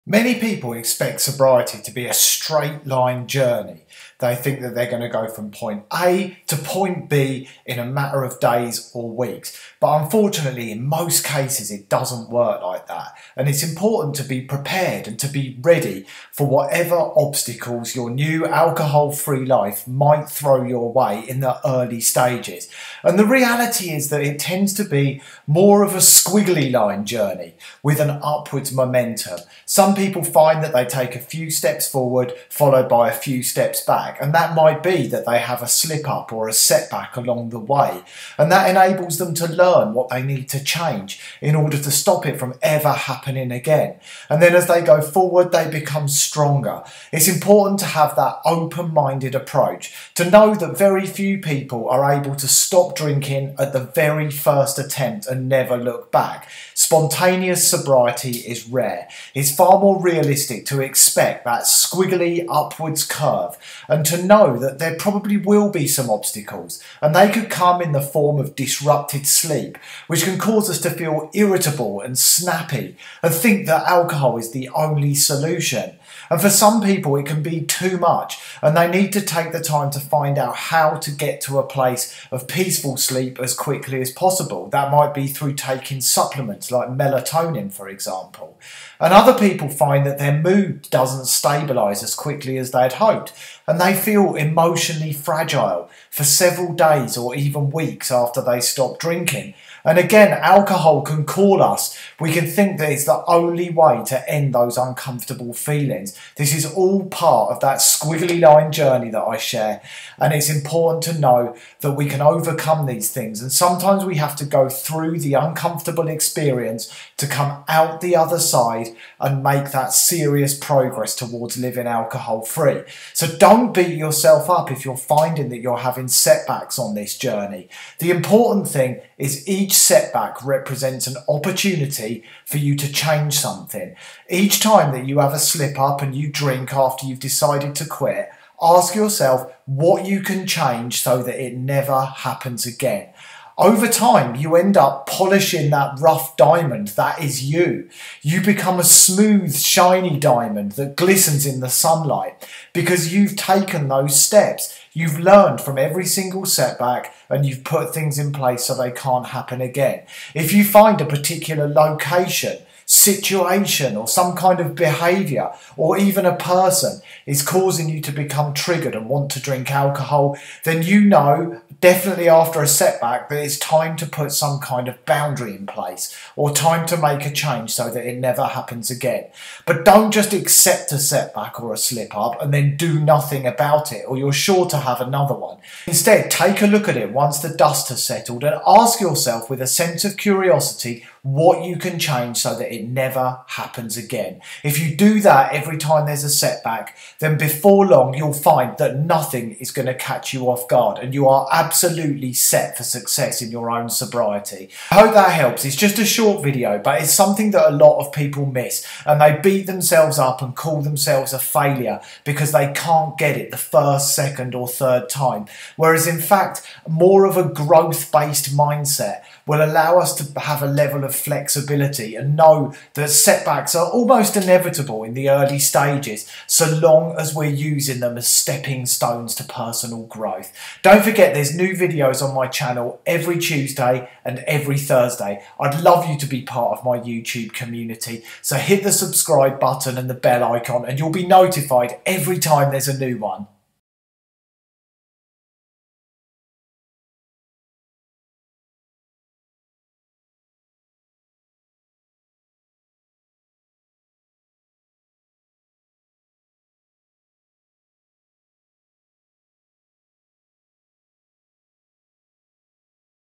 The reality of stopping drinking alcohol
Sobriety is not a straight line journey yet many people expect to be a simple case of going from point A to point B. This video will allow you to understand the reality and what to expect.

If you are able to set your expectations realistically you will have a far higher likelihood of success when it comes to stopping drinking, rather than returning to day one over and over.

If you need help to stop drinking check out my programme

Simon Chapple
The Quit Alcohol Coach